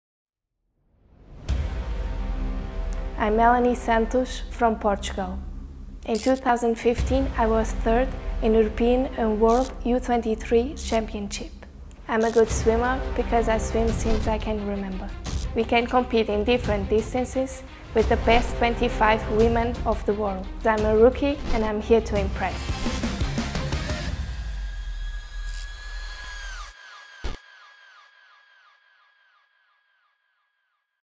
Melanie Santos
Melanie Santos is the next wave of talent from Portugal, taking the bronze medal at the 2015 U23 world championships the same weekend Portugal’s Olympic star Vanessa Fernandes entered the ITU Hall of Fame. While Melanie is still cutting her teeth in elite Olympic distance competition, her youth and speed have her coming out among the top athletes in the sprint distance. Can this translate to her Super League Triathlon performance?